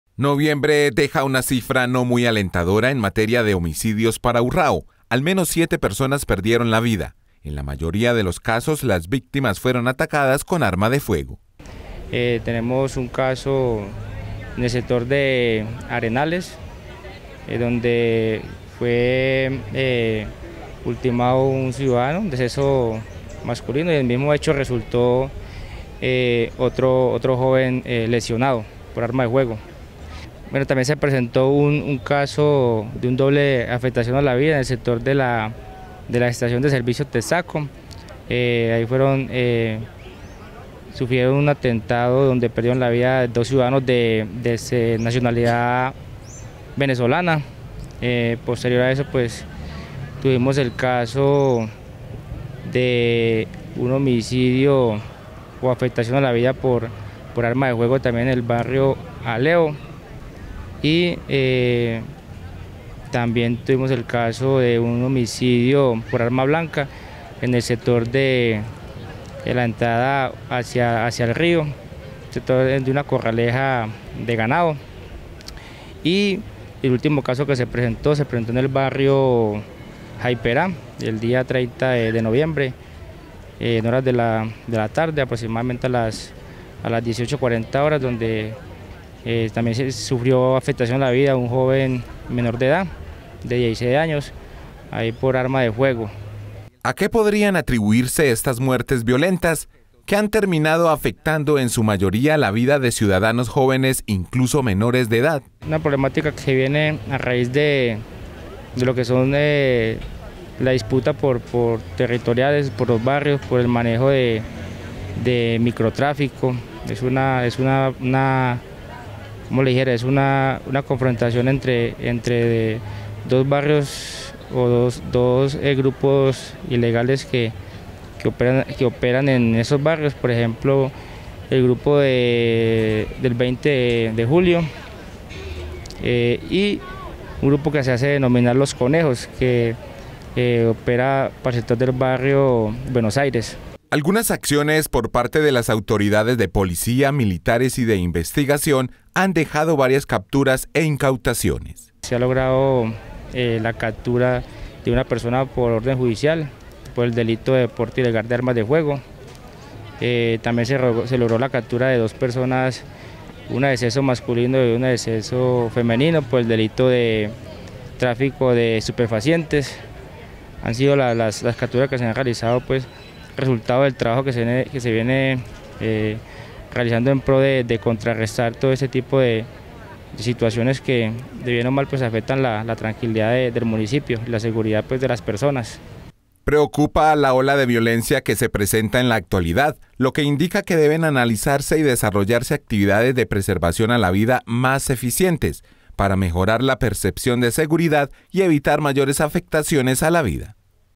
Noviembre uno de los meses más violentos
Noviembre uno de los meses más violentos del 2022, al menos 7 homicidios se presentaron en la localidad presuntamente por disputas del microtráfico, al mismo tiempo las autoridades también reportan algunos resultados operativos que permitieron capturas e incautaciones.